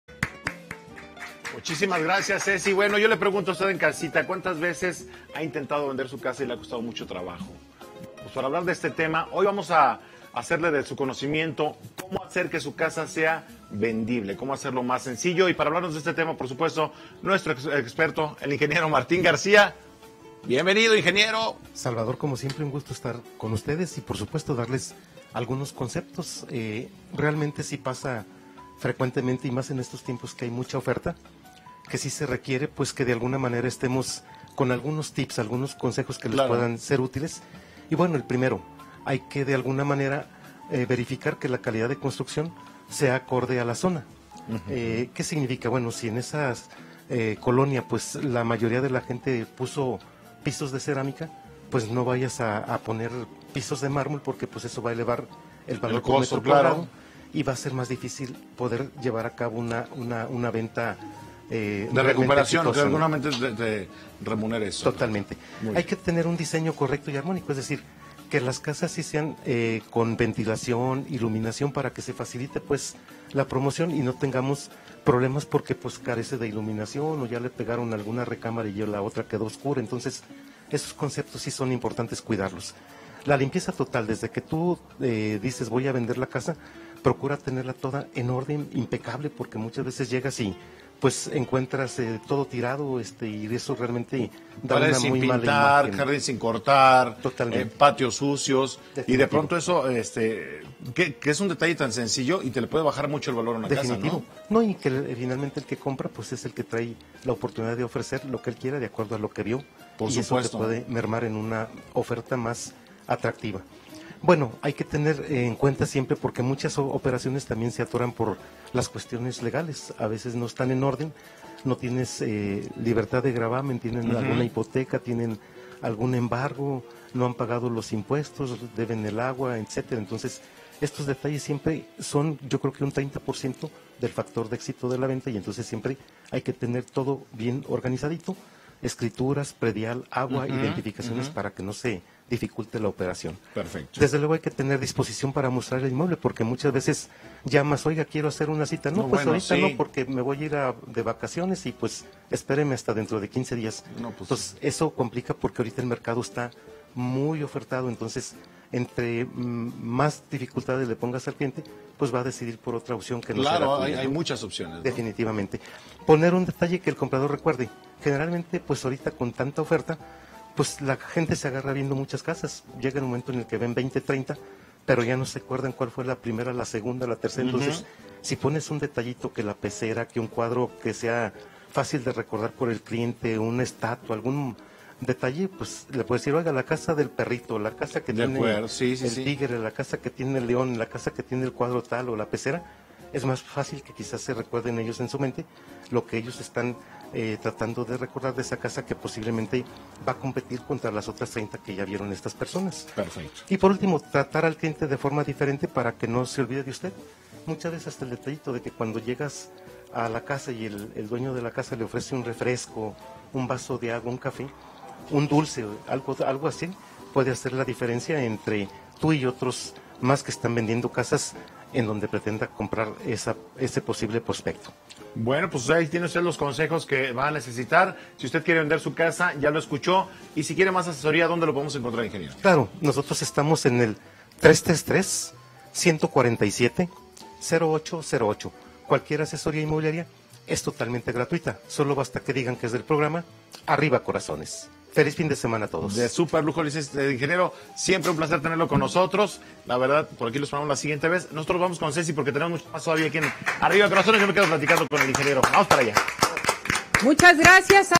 ¡La mejor estrategia para vender tu casa! 🏡 | Bienes raíces
En la sección de # Asesoría Inmobiliaria, el Ing. Martín García, Director de Mundo Firme, nos brinda algunos consejos para hacer una casa vendible.